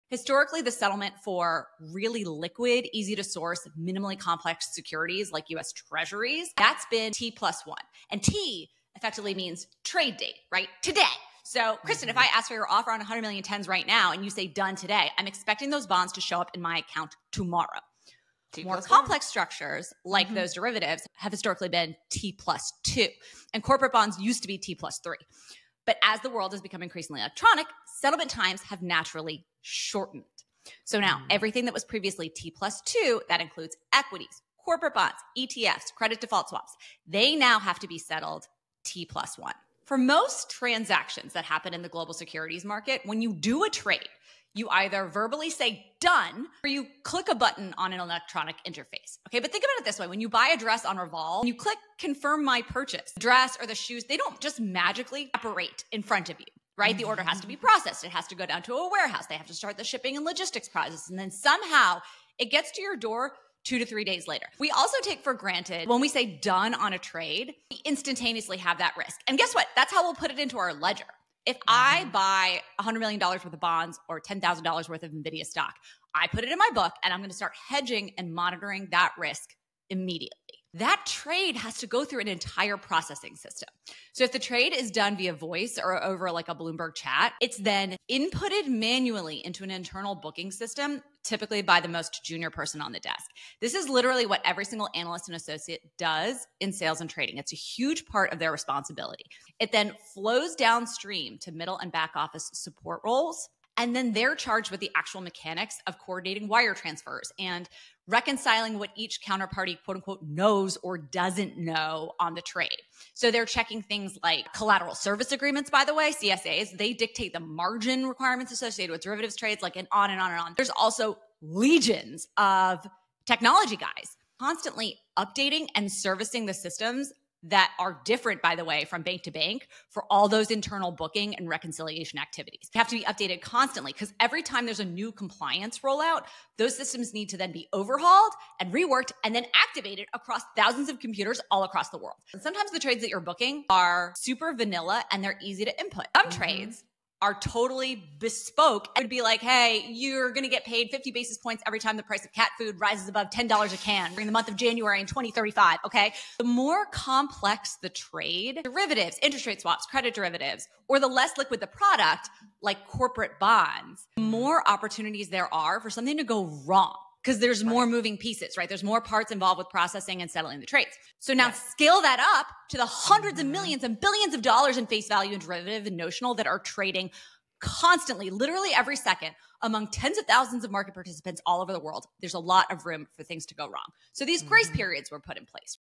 T+1 Settlement Change Explained!
A quick rundown to help you understand the T+1 settlement rule change implemented on May 28th, 2024.  Essential viewing for anyone trading equities or fixed income securities, and ESPECIALLY critical for anyone working in a middle office or back office support role, technology, or compliance at an investment bank, broker dealer, asset manager, hedge fund, and more!